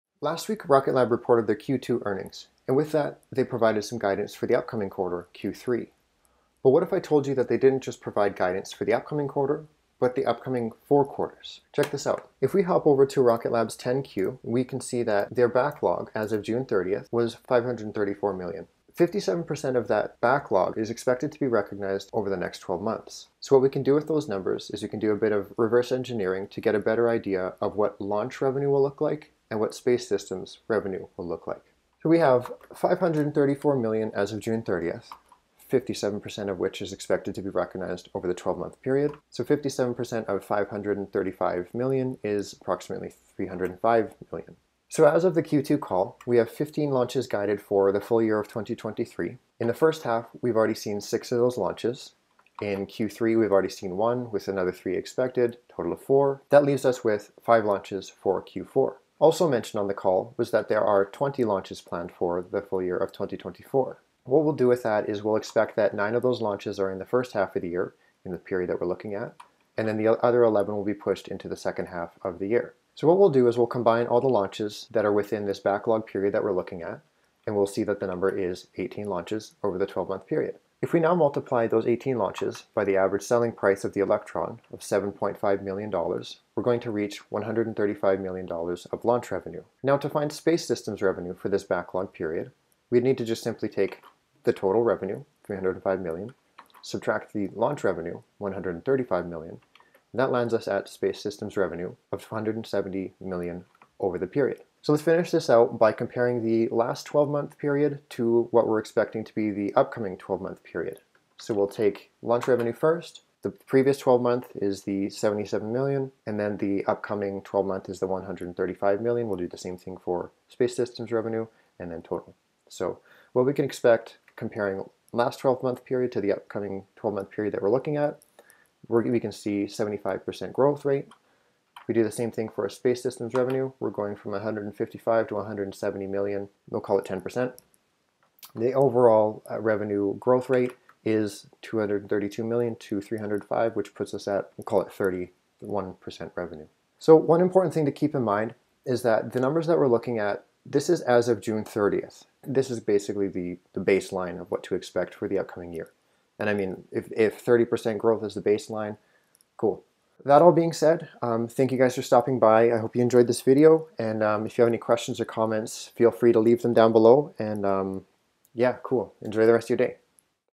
Rocket Lab Q2 2023 - What Everyone Missed
Rocket Lab provided their Q2 results, Q3 guidance, and a whole lot more!     Let's take a look.